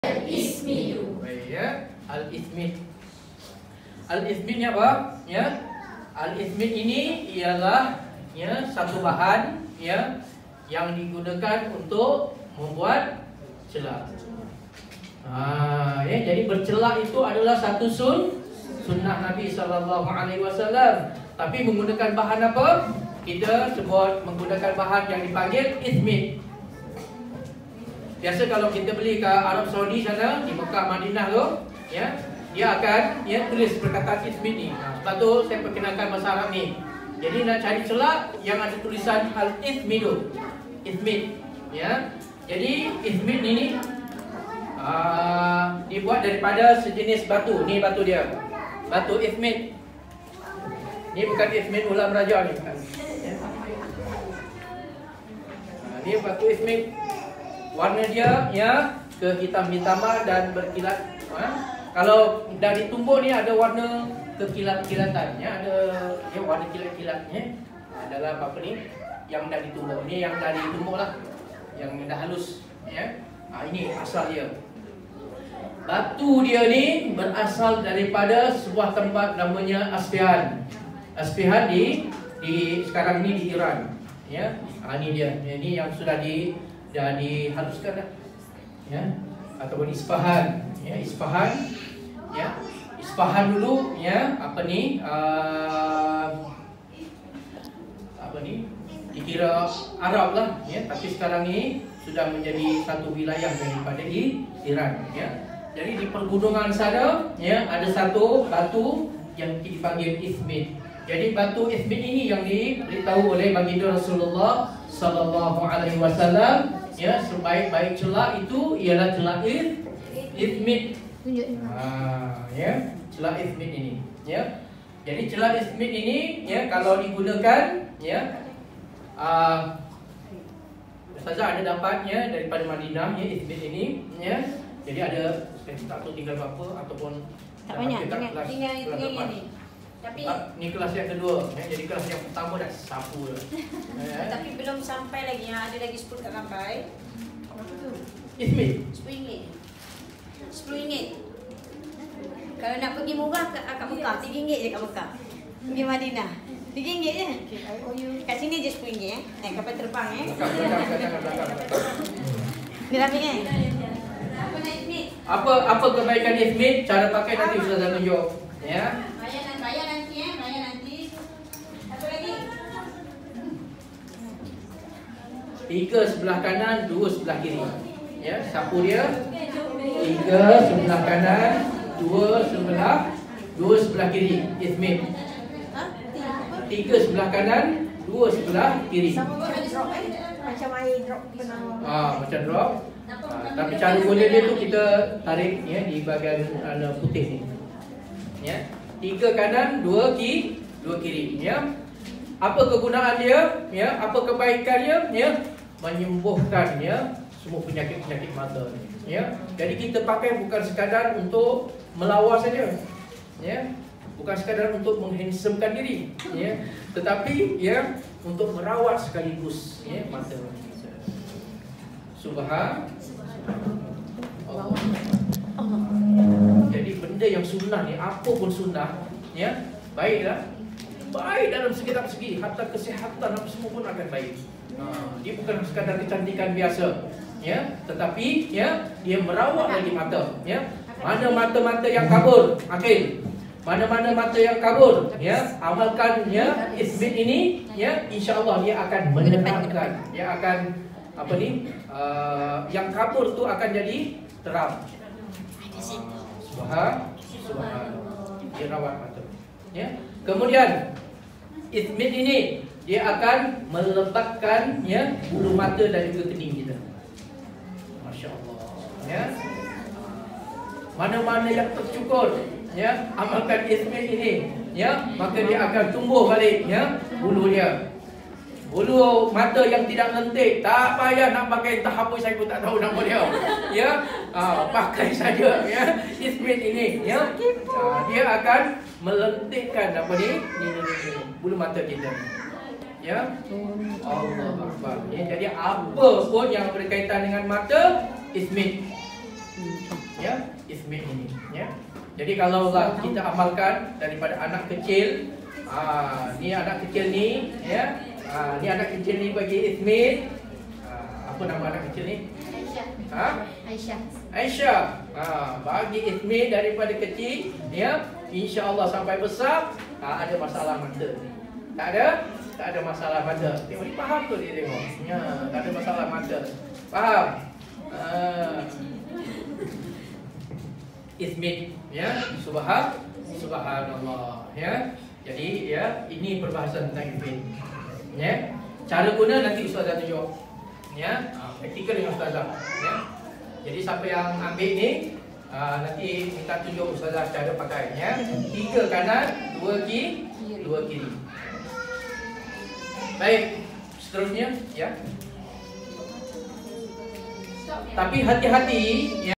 ITHMID amalan celak ikut sunnah banyak kebaikan mata jom dengar sampai habis drpd USTAZ ZAIDI
Tahukah ITHMID ini adalah rawatan sunnah untuk atasi masalah mata dan ada kaitan dengan sistem jantung -apapun dengar sampai habis dan belajar datang kelas diadakan secara percuma oleh YDP PERSATUAN PERUBATAN SUNNAH USTAZ HJ ZAIDI BIN HAJI TOHIRIN